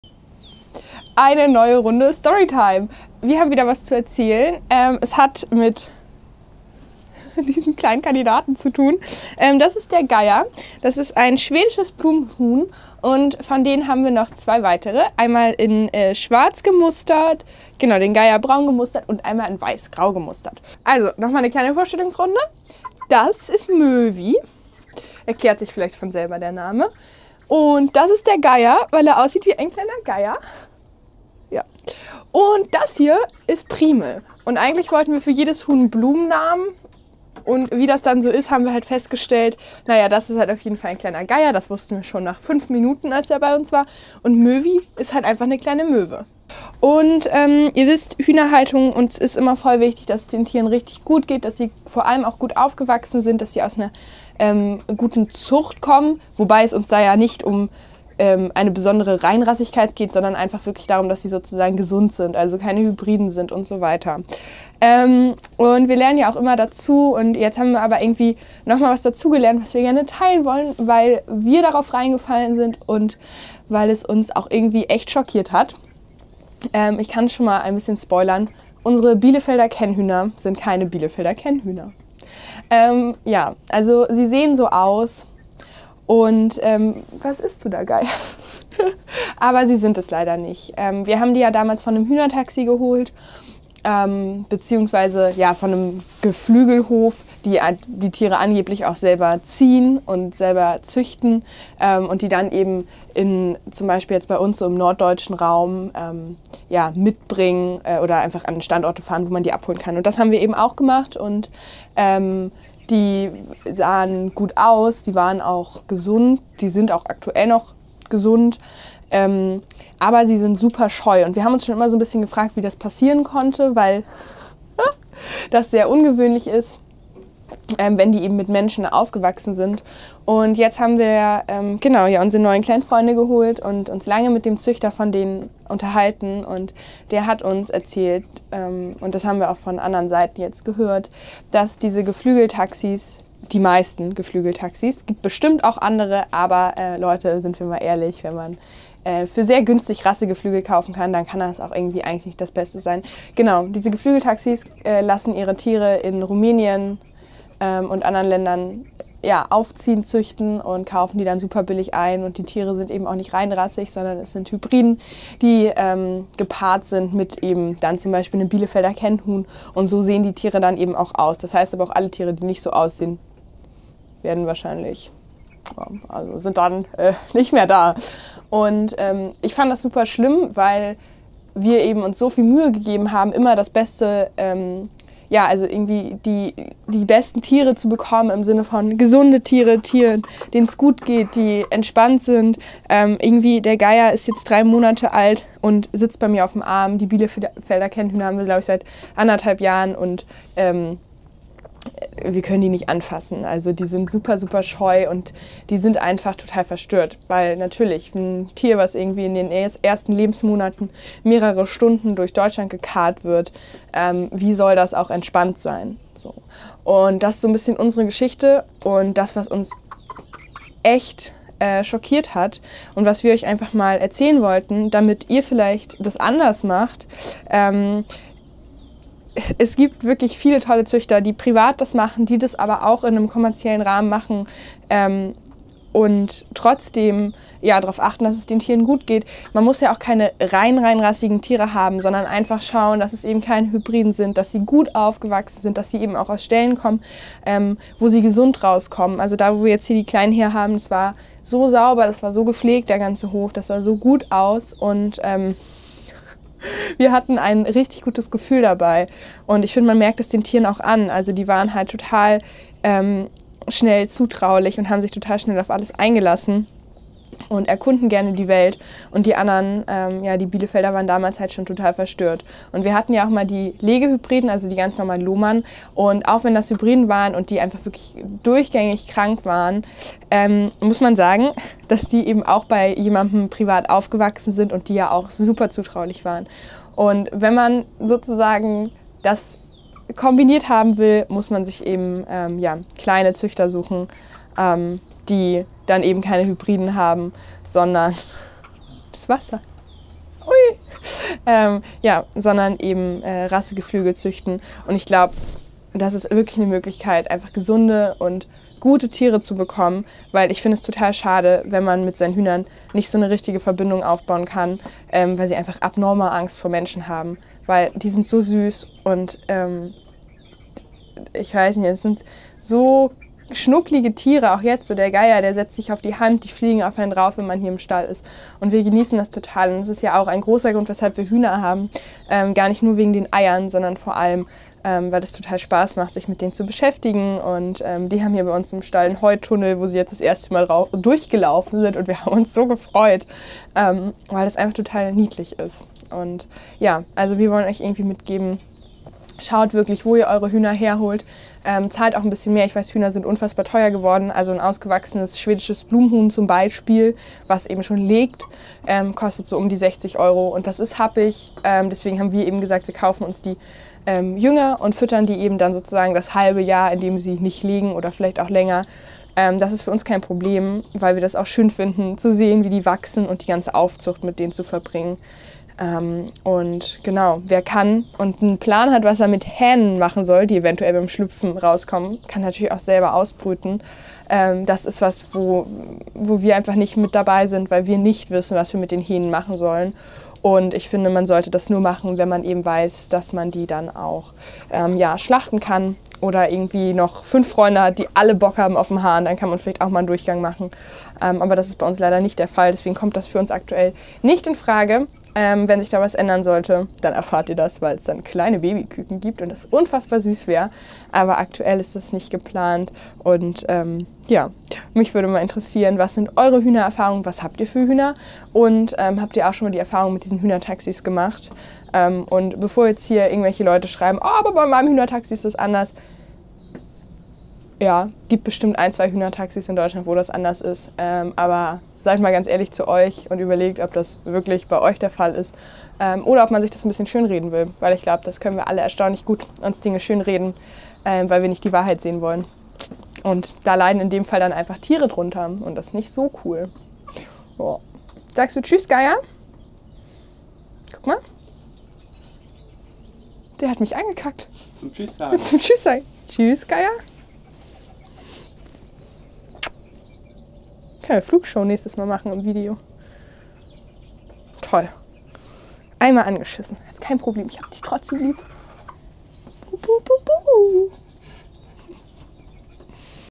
Unsere Bielefelder Kennhühner sind Hybriden 😱
Wie, was , warum...
Kurzes Storytime weshalb Hühner kaufen eine richtige Challenge ist und wir sie gleich zweimal verloren haben!